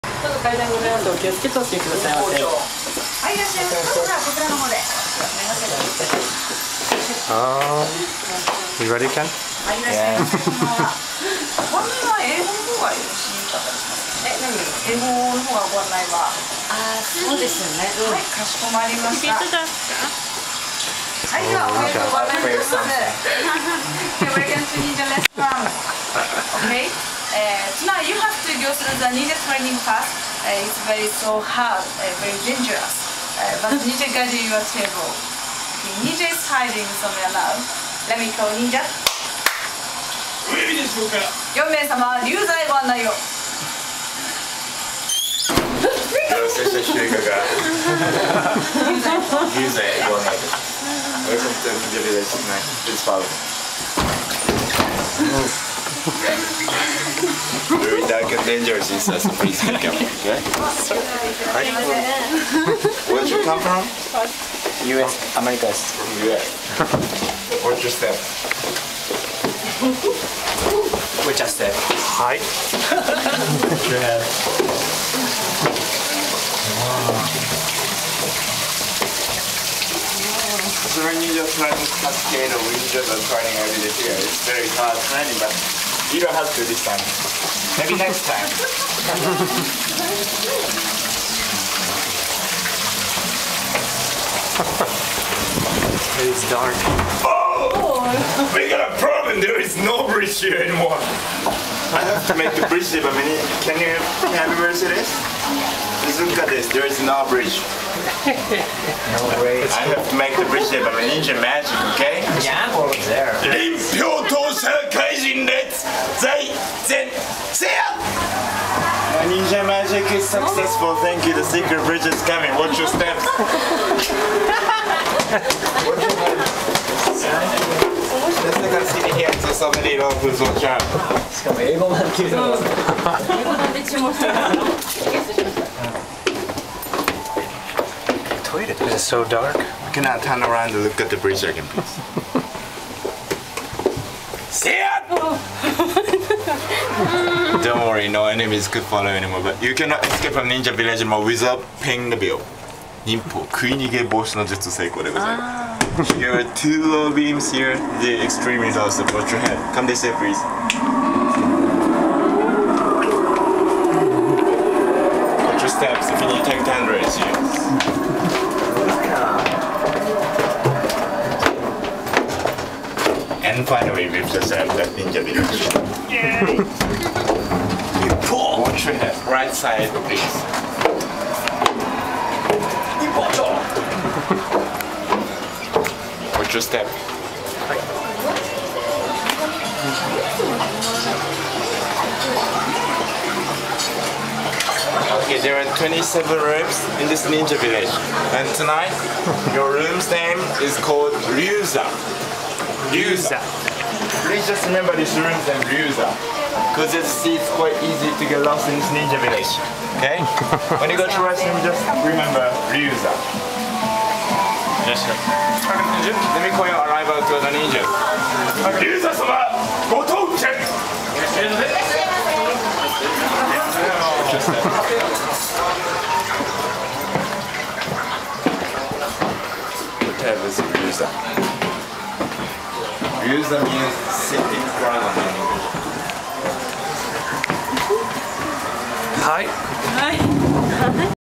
Entering Ninja Akasaka Restaurant
We had to make our way through some dark pathways to get to our table.  Was awesome.  Some of the video was a little dark but you can hear our ninja talking throughout.  This place is awesome.  A little pricey but well worth it.

You can kinda see what is going on.  We had to do a little ninja training to get to our table.